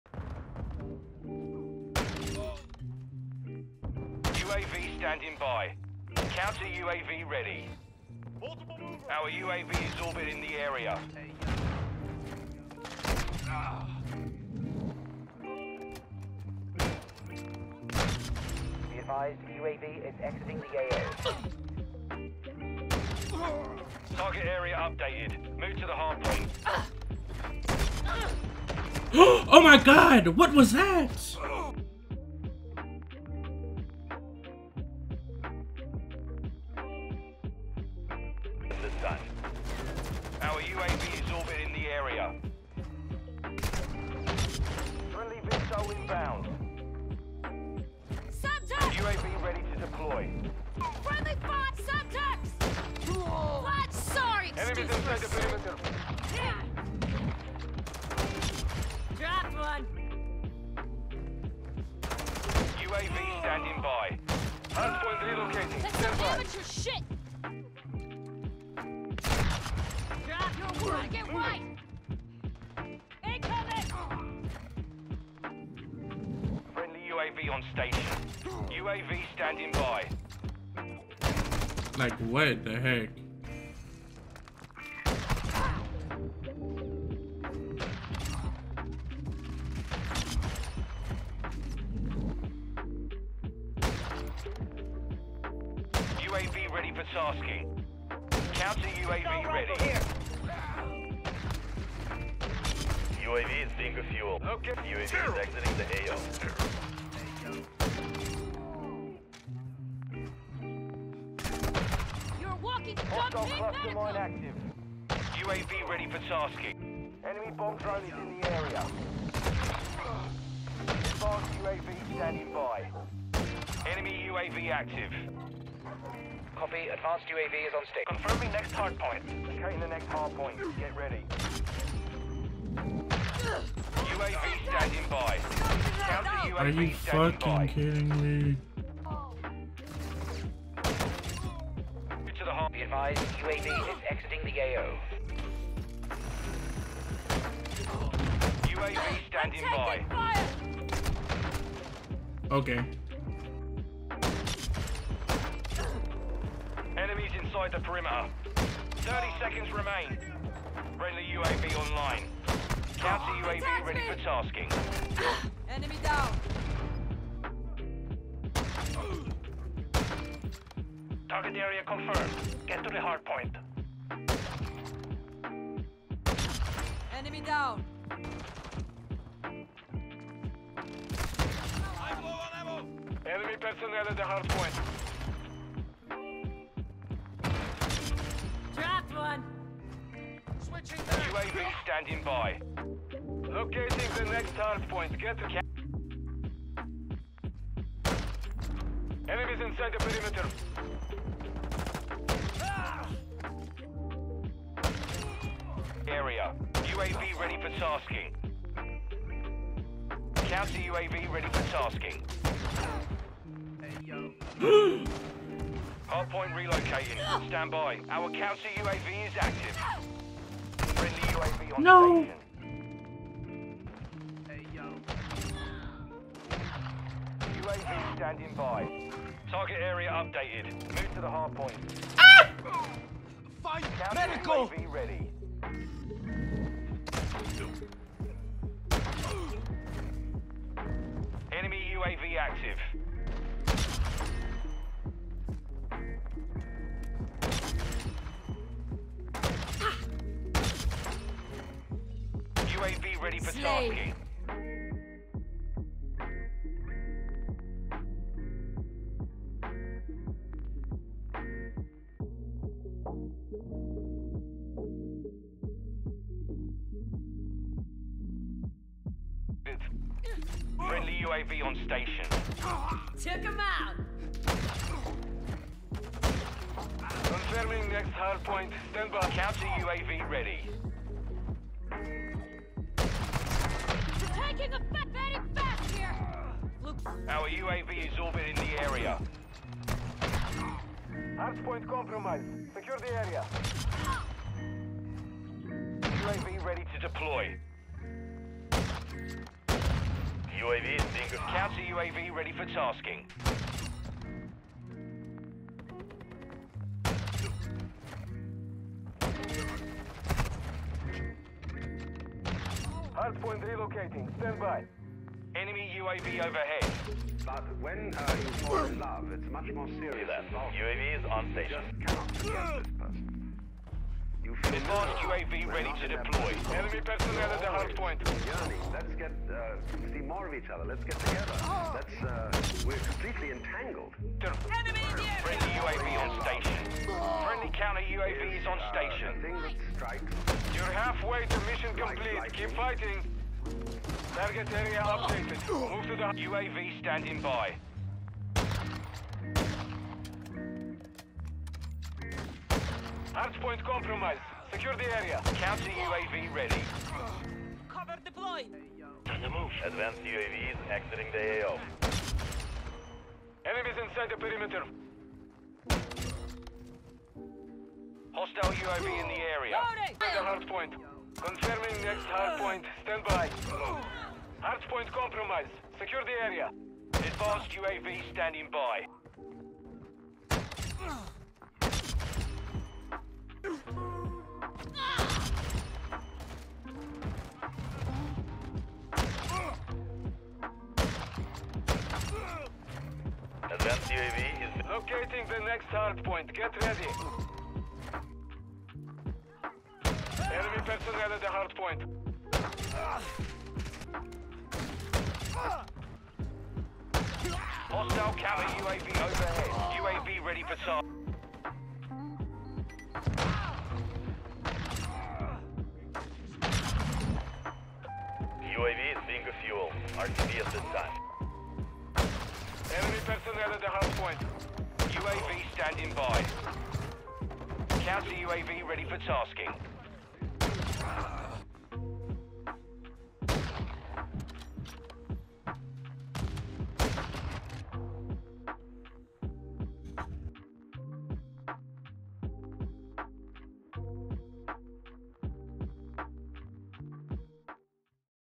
The SAB-50 Is OVERPOWERED with the SPX Scope! - Best SAB-50 Class Setup!
This: The SAB-50 Is UNSTOPPABLE with the SPX Scope! - Best SAB-50 Class Setup!

What’s going on guys, Stoopid here bringing you guys an SAB-50 class setup video!

Chapters:
00:00 - Intro Clips
00:32 - Project VII
00:39 - Clips & Highlights
04:15 - Class Setup
04:30 - 80 Kill Gameplay